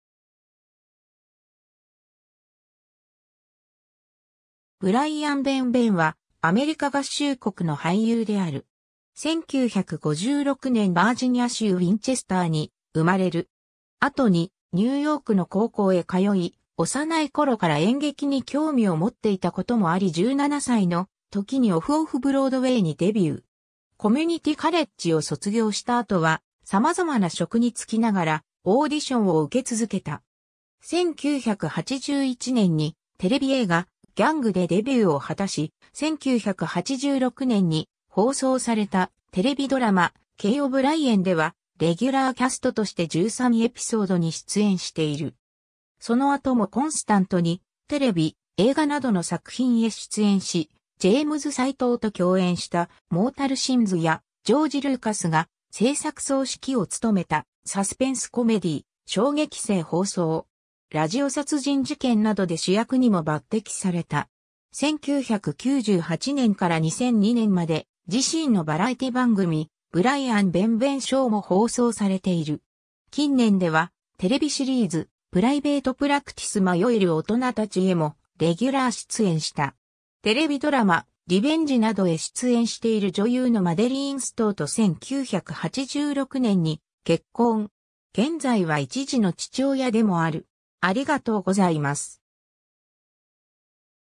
ブライアン・ベンベン
ブライアン・ベンベン（Brian Benben, 1956年6月18日 - ）は、アメリカ合衆国の俳優である。
 1956年バージニア州ウィンチェスターに生まれる。
後にニューヨークの高校へ通い、幼い頃から演劇に興味を持っていたこともあり17歳の時にオフ・オフ・ブロードウェイにデビュー。
コミュニティ・カレッジを卒業した後は、様々な職に就きながらオーディションを受け続けた。
1981年にテレビ映画『ギャング』でデビューを果たし、1986年に放送されたテレビドラマ『Kay O'Brien』ではレギュラーキャストとして13エピソードに出演している。
その後もコンスタントにテレビ、映画などの作品へ出演し、ジェームズ・サイトウと競演した『Mortal Sins』やジョージ・ルーカスが製作総指揮を務めたサスペンス・コメディ『笑撃生放送! ラジオ殺人事件』などで主役にも抜擢された。
1998年から2002年まで、自身のバラエティ番組『ブライアン・ベンベン・ショー』も放送されている。
近年ではテレビシリーズ『プライベート・プラクティス 迷えるオトナたち』へもレギュラー出演した。
 テレビドラマ『リベンジ』などへ出演している女優のマデリーン・ストウと1986年に結婚。
現在は1児の父親でもある。